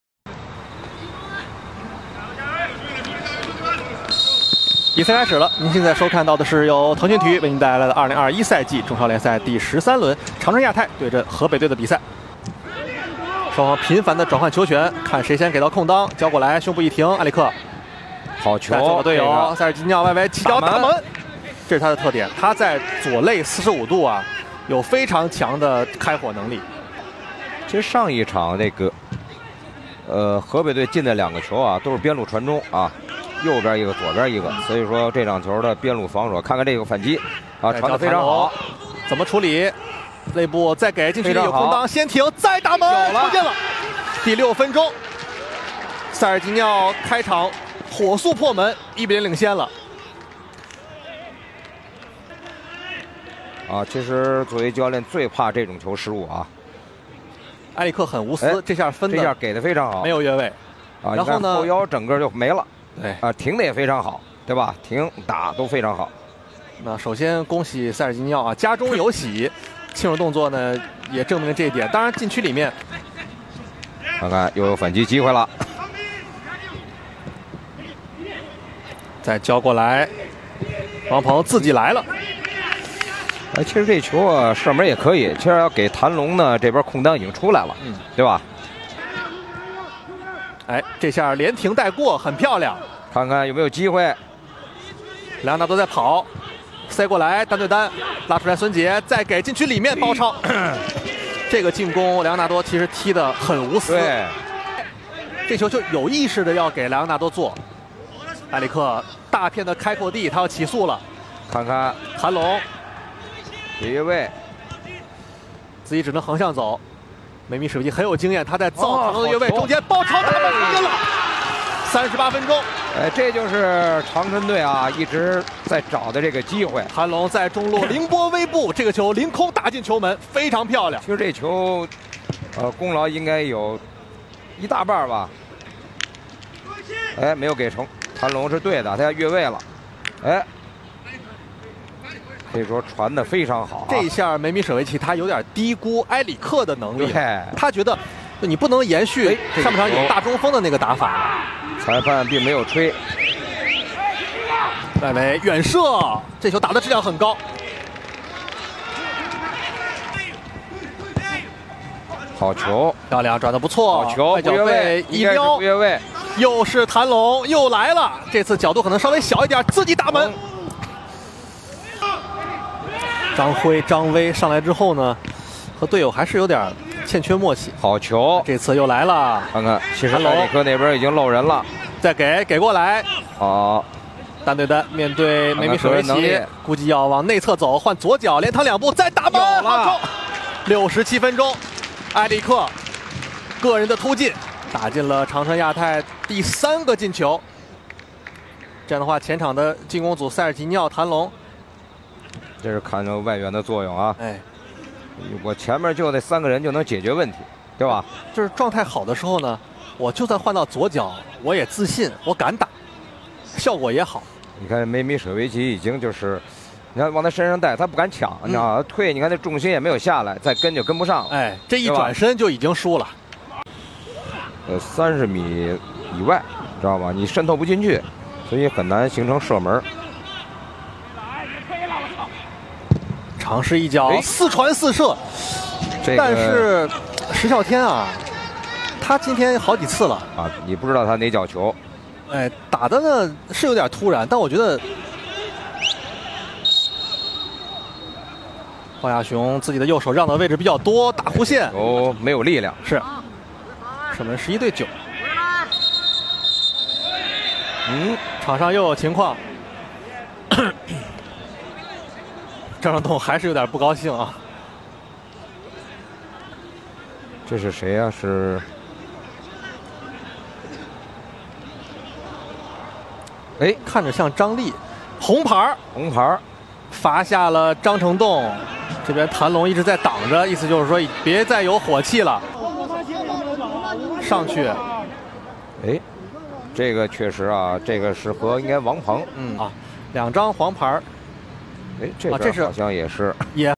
2021中超联赛第13轮:谭龙1球2助攻埃里克传射 长春亚泰3-0河北队
北京时间8月9日20:30，中超第13轮，长春亚泰对阵河北队。上半场，谭龙开场6分钟助攻塞尔吉尼奥破门，随后他接埃里克传中凌空垫射建功；下半场，亚泰数次利用反击威胁河北队防线，谭龙助攻埃里克锁定胜局，张呈栋和王鹏补时各自吃到一张红牌。最终，亚泰3-0击败河北队，队史首次击败对手的同时锁定争冠组名额。

长春亚泰：28-石笑天、4-奥克雷、5-孙捷（55' 39-毛开宇）、21-崔麒、25-王鹏、31-饶伟辉、10-塞尔吉尼奥（70' 17-陈柏良）、11-埃里克（70' 26-杨超声）、15-迪力依米提-土地（U23）（46' 20-张宇峰（U23））、32-李光文（80' 27-张力）、29-谭龙

河北队：30-鲍亚雄、5-梅米舍维奇、21-崔麟、28-张呈栋、32-丁海峰、6-塞尔纳斯（83' 35-金强）、13-尹鸿博、15-王秋明（83' 17-宋鑫涛（U23））、23-席尔瓦（46' 38-张威（U23））、25-廖伟（U23）（46' 11-胡人天）、45-莱昂纳多（46' 40-张辉（U23））